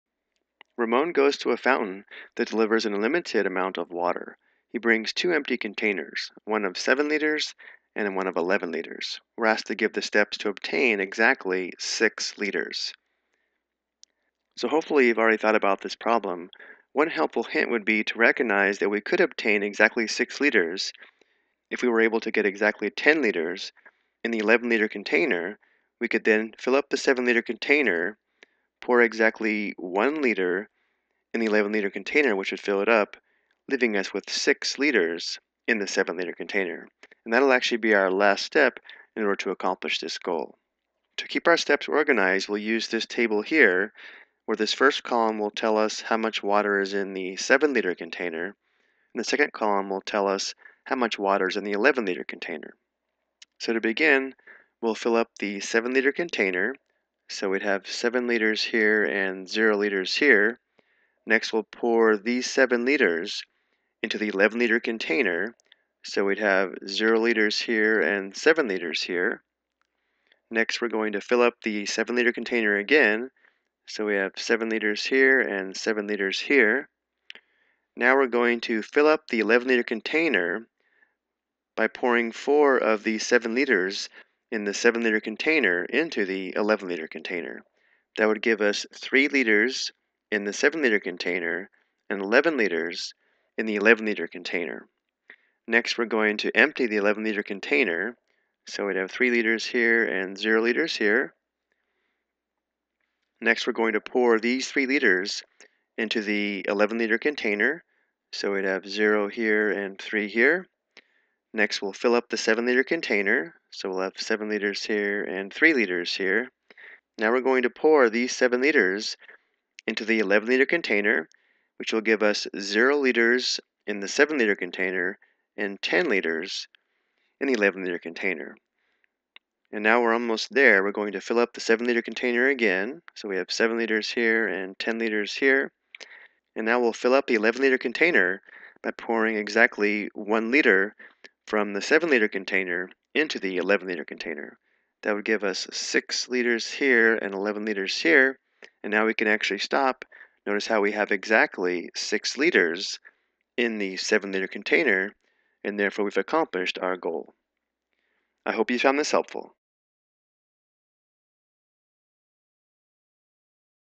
Ex: Get 6 Liters Using Only 7 Liters and 11 Liters
This video explains how to obtain exactly 6 liters of water only using a 7 liter and 11 liter container.